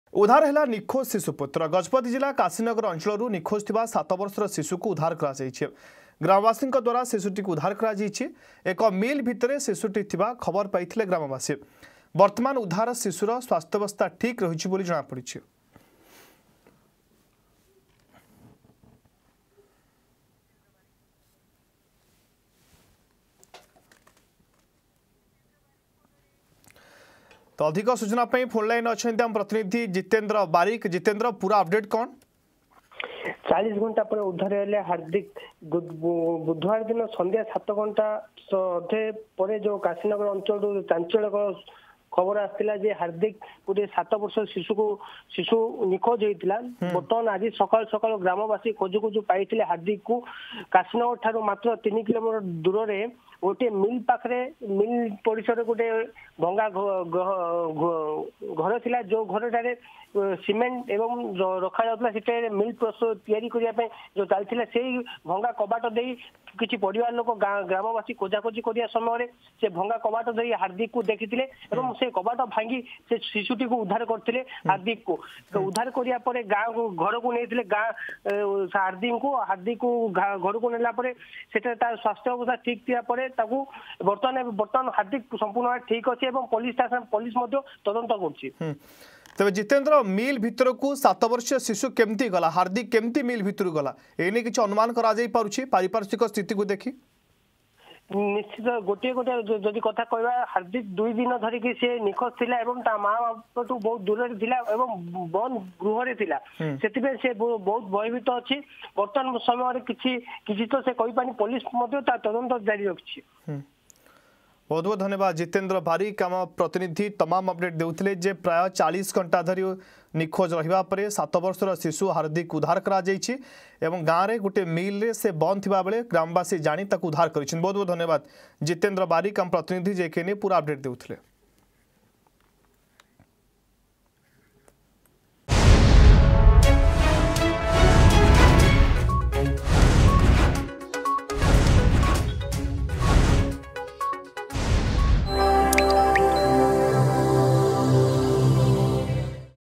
ଉଦ୍ଧାର ହେଲା ନିଖୋଜ ଶିଶୁପୁତ୍ର ....
ଉଦ୍ଧାର ହେଲା ନିଖୋଜ ଶିଶୁପୁତ୍ର । ଗଜପତି ଜିଲ୍ଲା କାଶିନଗର ଅଞ୍ଚଳରୁ ନିଖୋଜ ଥିବା ୭ ବର୍ଷ ଶିଶୁ ଉଦ୍ଧାର କରାଯାଇଛି । ଗ୍ରାମବାସୀଙ୍କ ଦ୍ବାରା ଶିଶୁଟିକୁ ଉଦ୍ଧାର କରାଯାଇଛି ...